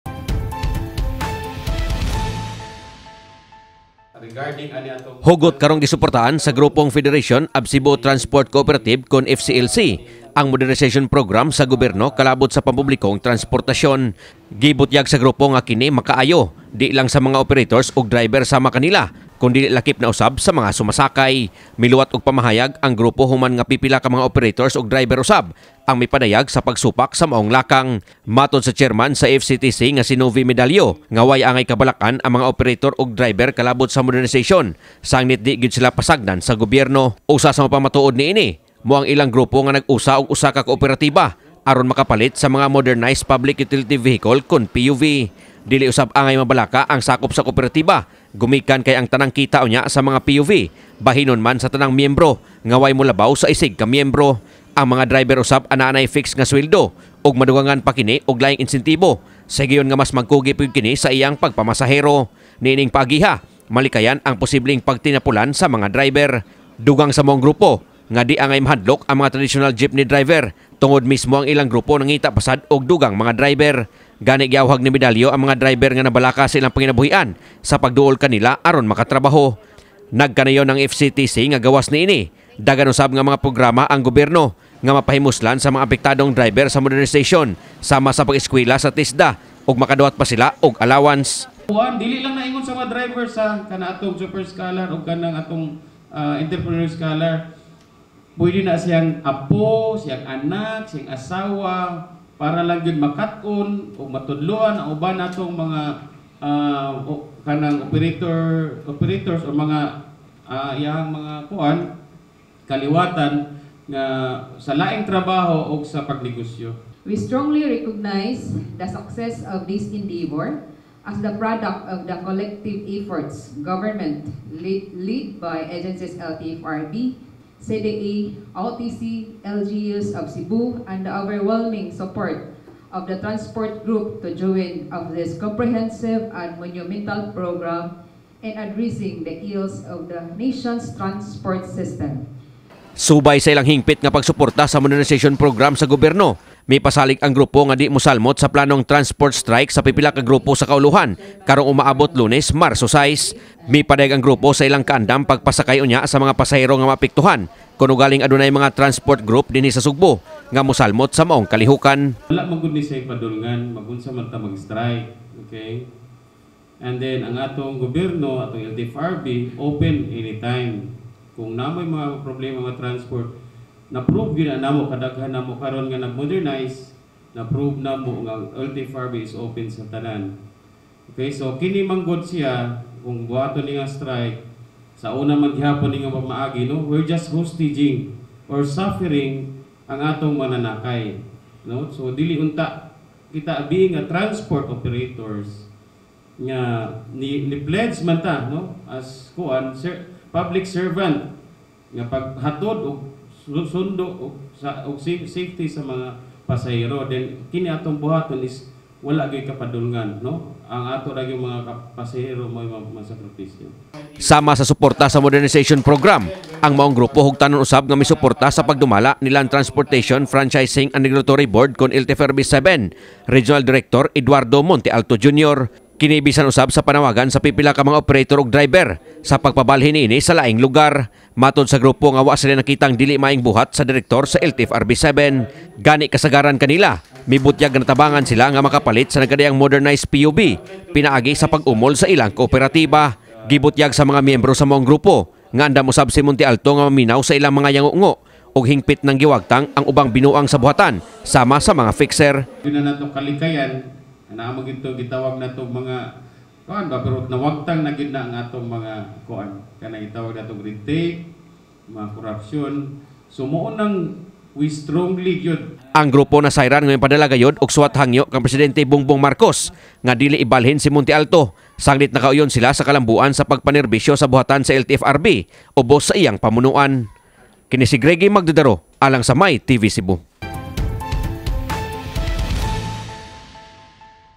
𝗠𝘆𝗧𝗩 𝗡𝗲𝘄𝘀 𝗨𝗽𝗱𝗮𝘁𝗲: 𝗠𝗶𝗽𝗮𝗱𝗮𝘆𝗮𝗴 𝘀𝗮 𝗽𝗮𝗴𝘀𝘂𝗽𝗼𝗿𝘁𝗮 𝘀𝗮 𝗺𝗼𝗱𝗲𝗿𝗻𝗶𝘇𝗮𝘁𝗶𝗼𝗻 𝗽𝗿𝗼𝗴𝗿𝗮𝗺 𝗮𝗹𝗮𝗻𝗴 𝘀𝗮 𝘁𝗿𝗮𝗻𝘀𝗽𝗼𝗿𝘁 𝘀𝘆𝘀𝘁𝗲𝗺
𝗠𝘆𝗧𝗩 𝗡𝗲𝘄𝘀 𝗨𝗽𝗱𝗮𝘁𝗲: 𝗠𝗶𝗽𝗮𝗱𝗮𝘆𝗮𝗴 𝘀𝗮 𝗽𝗮𝗴𝘀𝘂𝗽𝗼𝗿𝘁𝗮 𝘀𝗮 𝗺𝗼𝗱𝗲𝗿𝗻𝗶𝘇𝗮𝘁𝗶𝗼𝗻 𝗽𝗿𝗼𝗴𝗿𝗮𝗺 𝗮𝗹𝗮𝗻𝗴 𝘀𝗮 𝘁𝗿𝗮𝗻𝘀𝗽𝗼𝗿𝘁 𝘀𝘆𝘀𝘁𝗲𝗺 𝘀𝗮 𝗻𝗮𝘀𝘂𝗱  𝗮𝗻𝗴 𝗳𝗲𝗱𝗲𝗿𝗮𝘁𝗶𝗼 𝗼𝗳 𝗖𝗲𝗯𝘂 𝗧𝗿𝗮𝗻𝘀𝗽𝗼𝗿𝘁 𝗖𝗼𝗼𝗽𝗲𝗿𝗮𝘁𝗶𝘃𝗲 𝗱𝗶𝗻𝗵𝗶 𝘀𝗮 𝗦𝘂𝗴𝗯𝗼, 𝘁𝗮𝗹𝗶𝘄𝗮 𝘀𝗮 𝗽𝗮𝗴𝗽𝘂𝗴𝗼𝘀 𝘀𝗮 𝘂𝗯𝗮𝗻𝗴 𝗱𝗿𝗶𝘃𝗲𝗿𝘀 𝘂𝗴 𝗼𝗽𝗲𝗿𝗮𝘁𝗼𝗿𝘀 𝘀𝗮 𝗽𝗵𝗮𝘀𝗲-𝗼𝘂𝘁 𝘀𝗮 𝘁𝗿𝗮𝗱𝗶𝘁𝗶𝗼𝗻𝗮𝗹 𝗣𝗨𝗝.
Samtang mipasalig ang maong groupo motabang sila sa LTFRB kon madayon ang ipahigayong lapad nga transport strike sa Lunes.

Ania ang report ni Gregy Magdadaro.